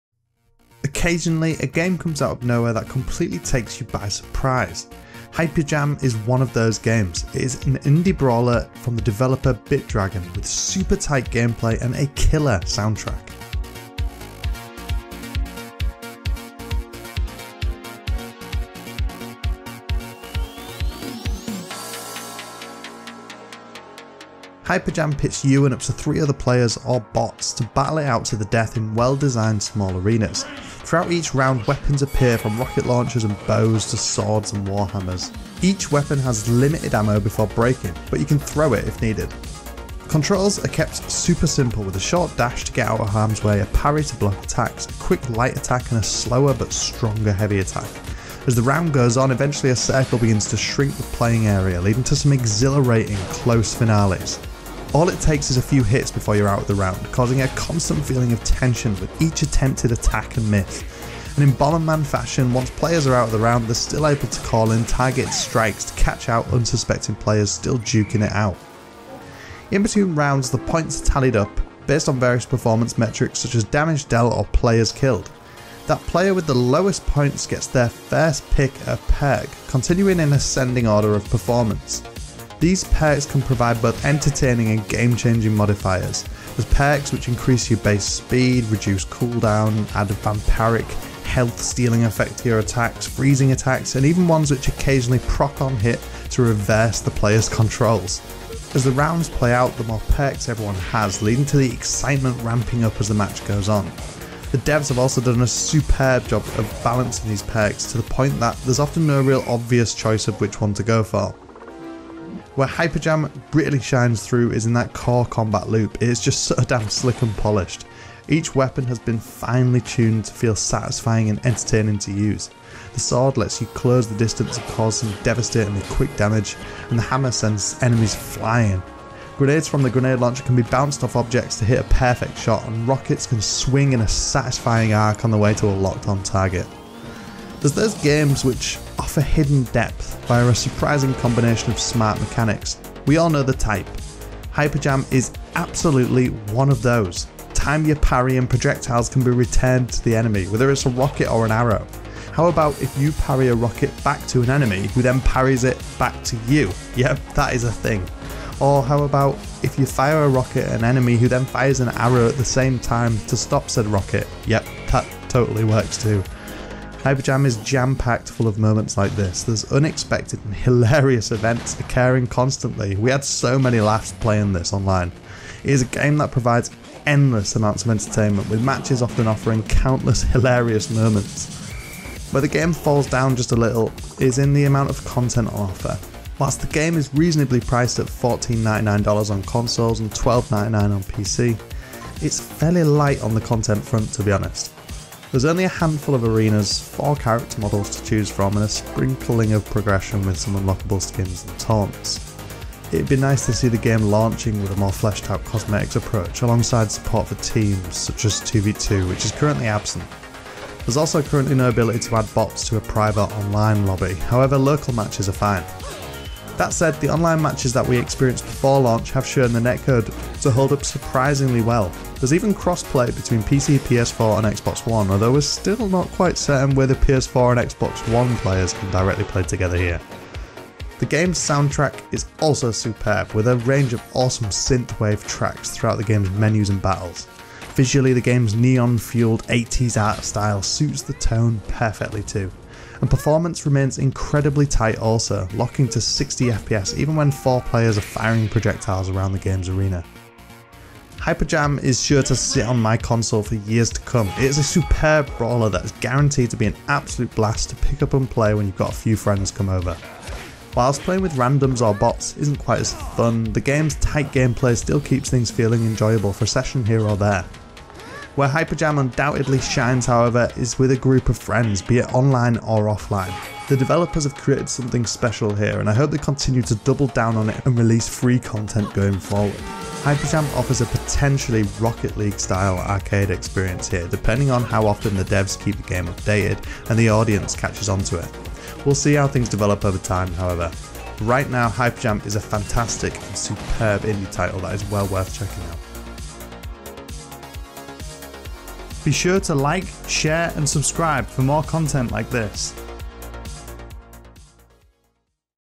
Hyper Jam Review - The Rocket League Of Brawlers? 🌴
Reviewed on - Xbox One X
Game provided by publisher for review

Follow Gaming LYF!